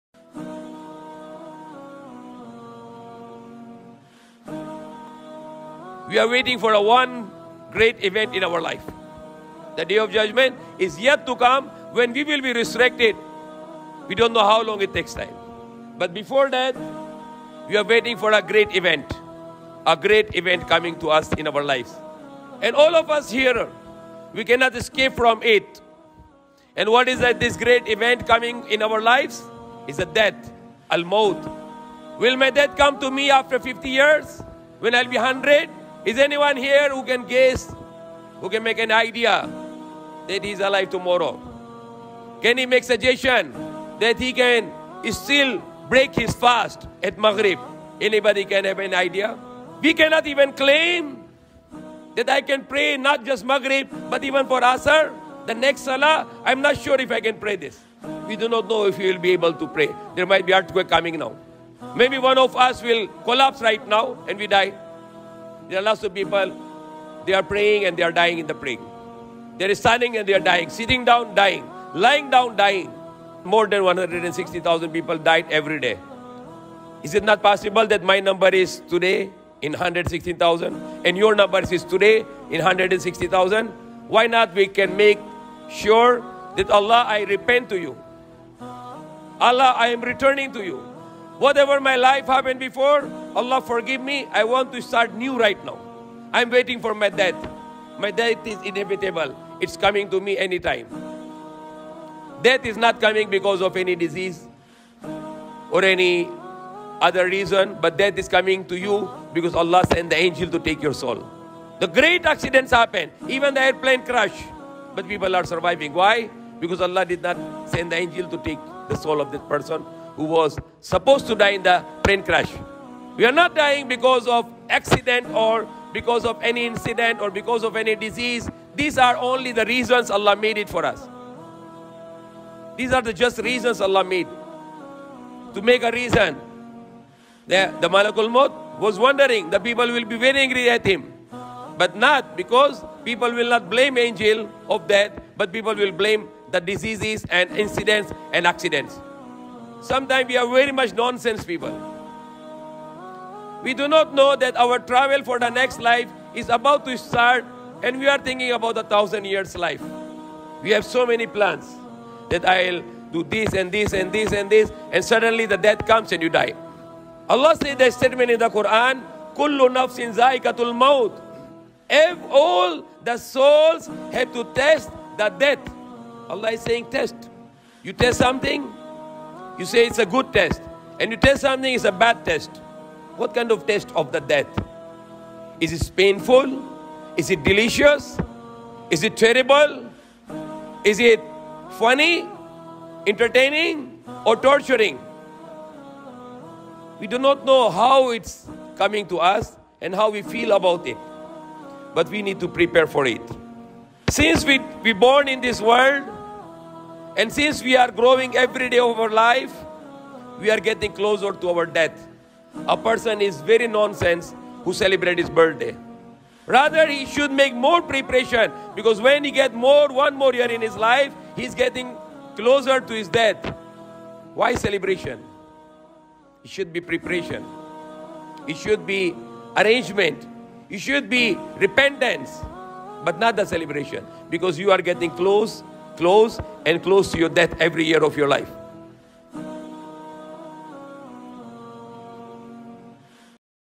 Death Is Near
Since we are born in this world and we are growing in this life, we are getting closer to our death. It is nonsense to celebrate your birthday. Rather, you should make more preparation because he one more year in his life, he is getting closer to his death.